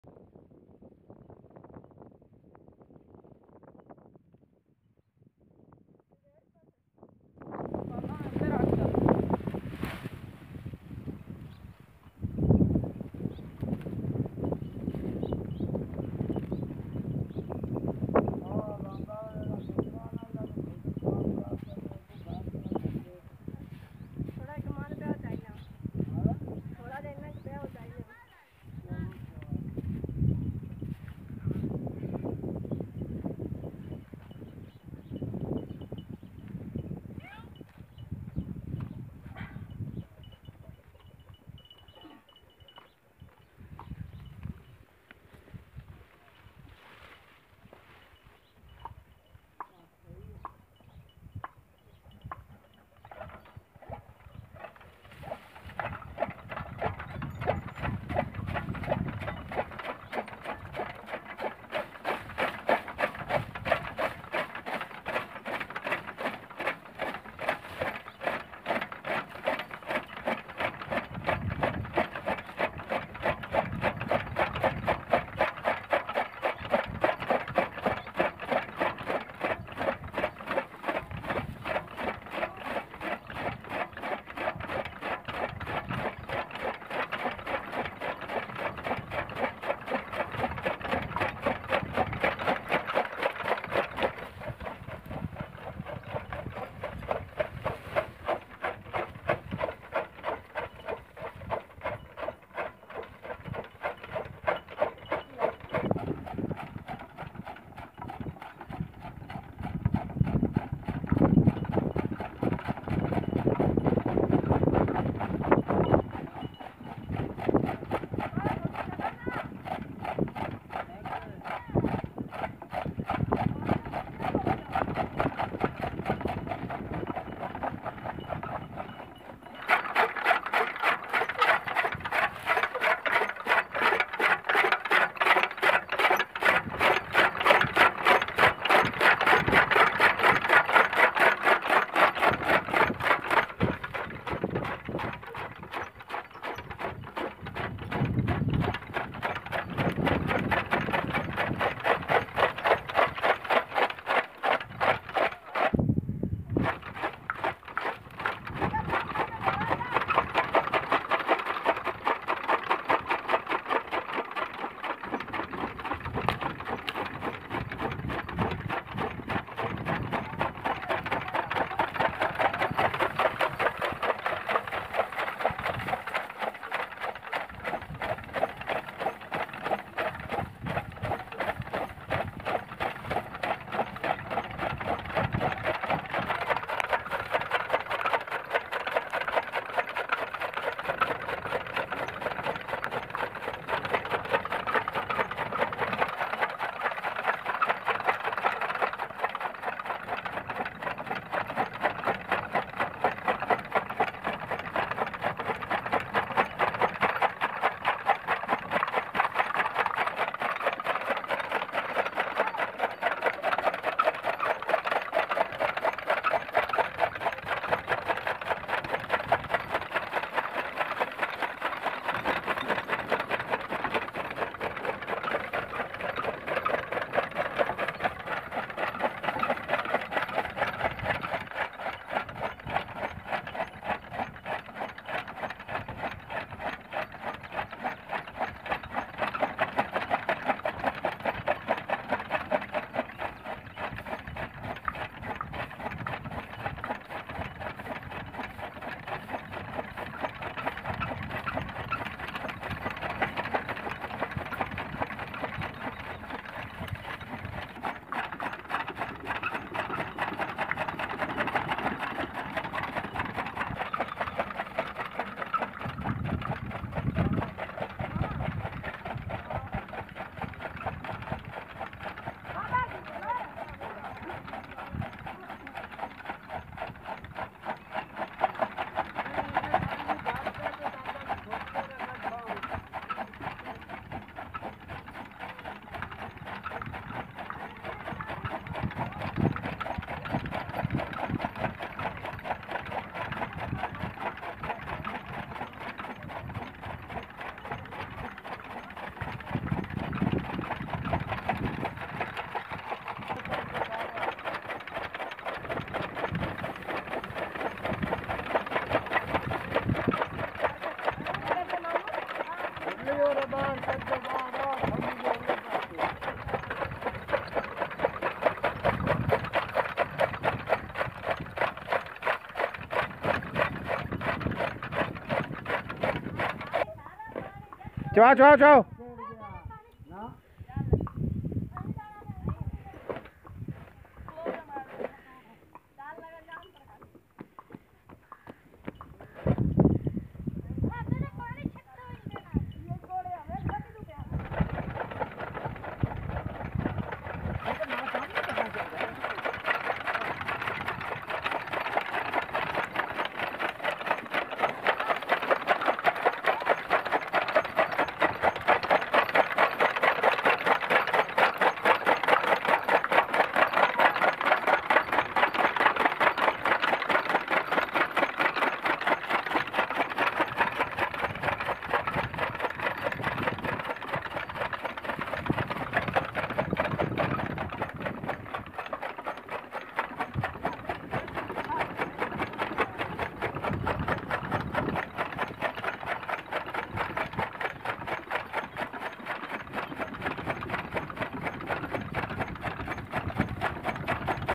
Amazing Pakistani Women Taking Care Farm Animals
This video shows hardworking and amazing Pakistani women who take care of farm animals and operating bull powered gear toka machine with their families. The women and girls work in farms alongside  with men. They manage the farms and look after the cattle by cutting fodder, milking, and cleaning. They also bring the fodder from the farms to the gear toka machine to chop. In many villages, farmers use old bull powered gear toka machines. In this video, you will see a bull-powered toka machine.